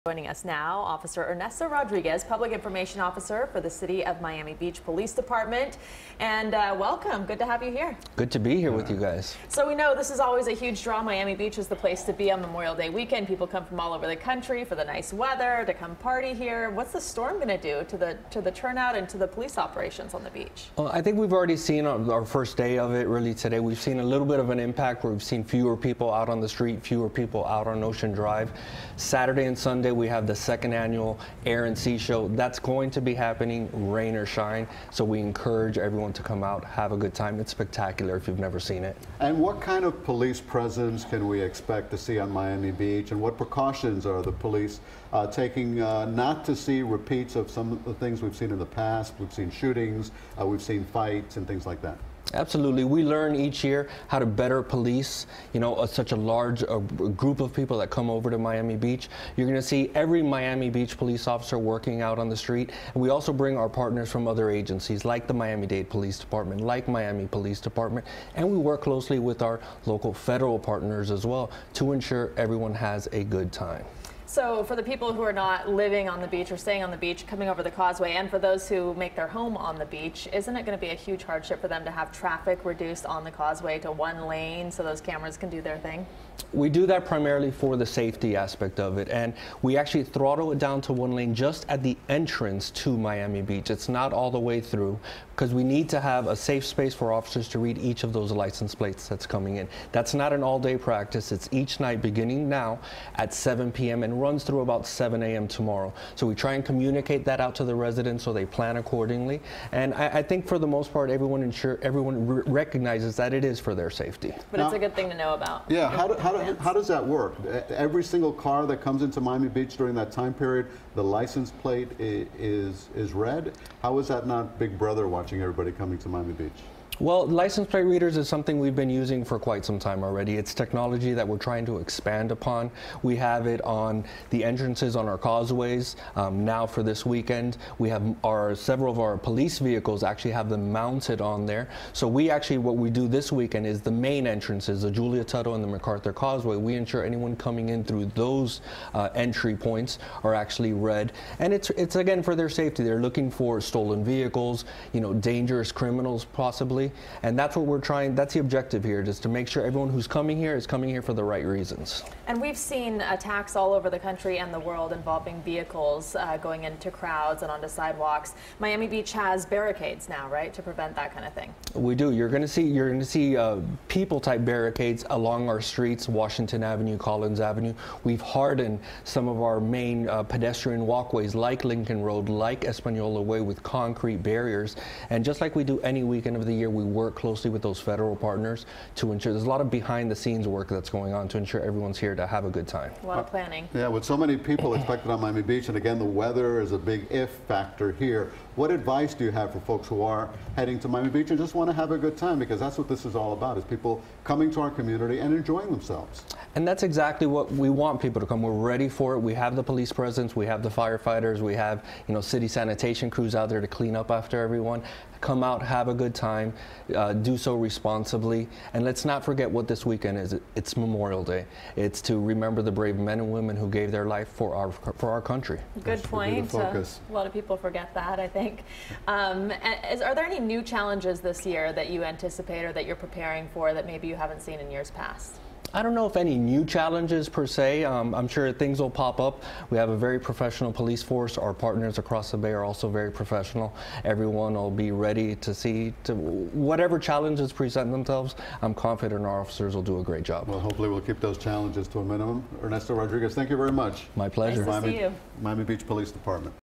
Miami Beach Police Ready For Memorial Day
Miami Beach Police Department Public Information Officer Ernesto Rodriguez on police preparations.